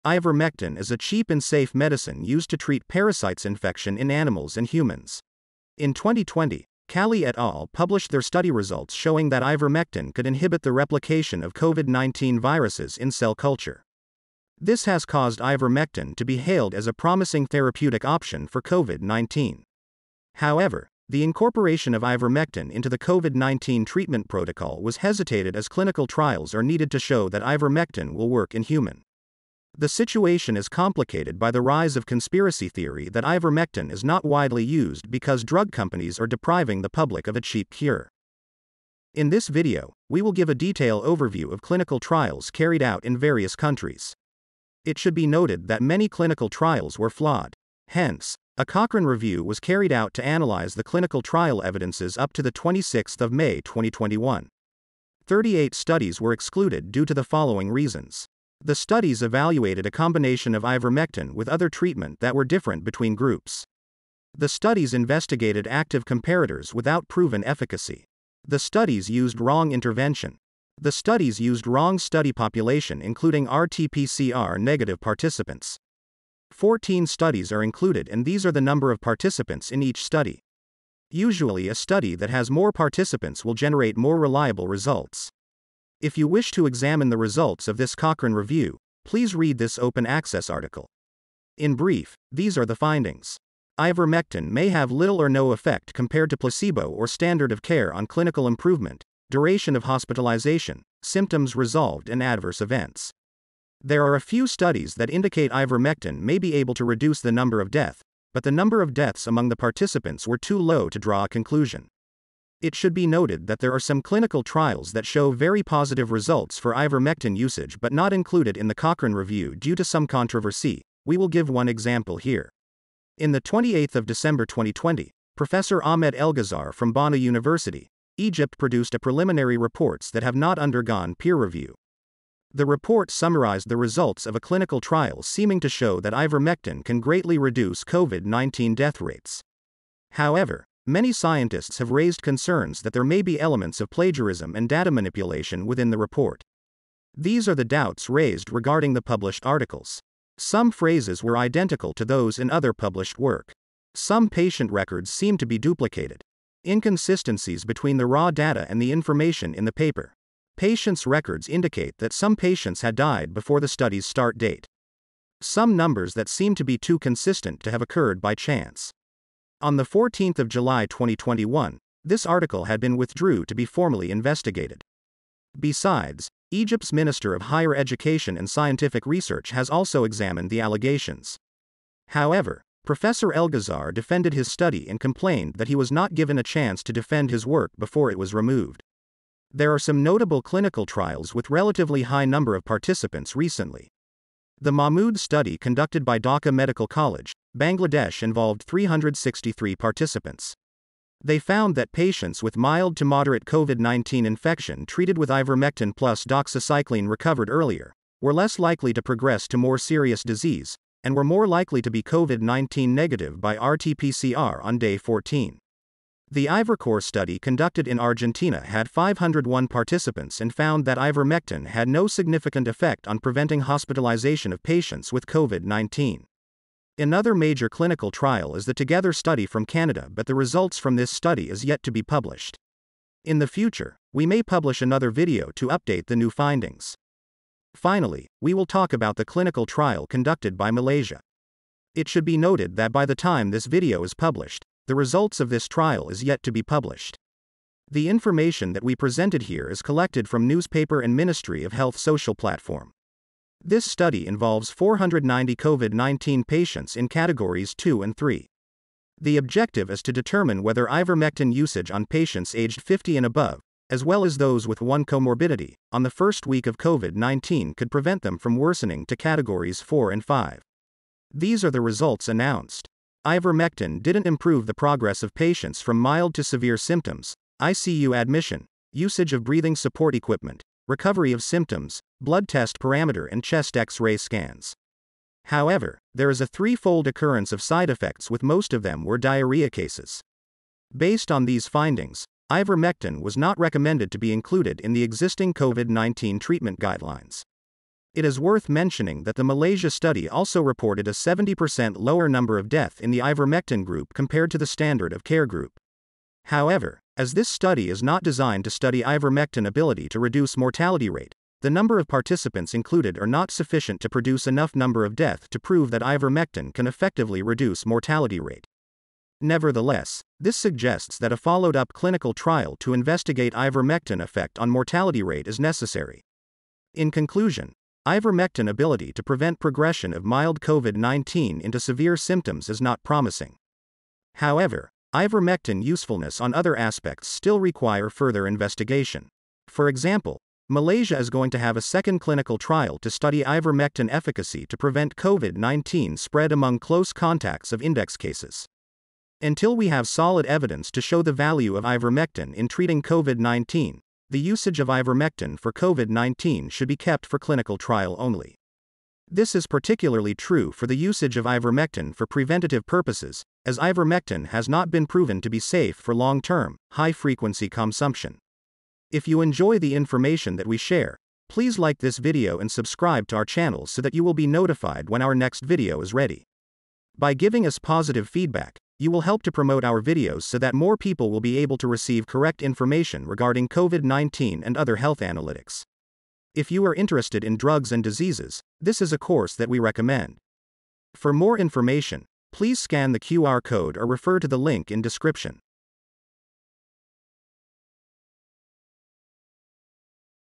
Ivermectin for COVID-19: Do we have the conclusion yet? 伊维菌素对新冠肺炎有效吗？
Ivermectin is a cheap and safe medicine used to treat parasites infection in animals and humans. However, there is hesitation in incorporating Ivermectin into the COVID-19 treatment protocol as clinical trials are needed to show that Ivermectin will work in human. In this video, we will give a detail overview of clinical trials carried out in various countries.

Ivermectin adalah ubat yang murah dan selamat untuk merawat jangkitan parasit pada haiwan dan manusia. Walau bagaimanapun, terdapat keraguan untuk memasukkan Ivermectin ke dalam protokol rawatan COVID-19 kerana ujian klinikal diperlukan untuk memastikan bahawa Ivermectin adalah berfungsi untuk mengubati COVID-19 pada manusia. Dalam video ini, kami akan memberikan gambaran terperinci tentang ujian klinikal yang dijalankan di pelbagai negara.

伊维菌素是一种廉价且安全的药物，用于治疗动物和人类的寄生虫感染。 然而，我们还不确定将伊维菌素纳入新冠肺炎治疗方案是否可行。所以我们需要临床试验来证明伊维菌素对人体有效。 在本视频中，我们将详细介绍在各个国家/地区进行的临床试验。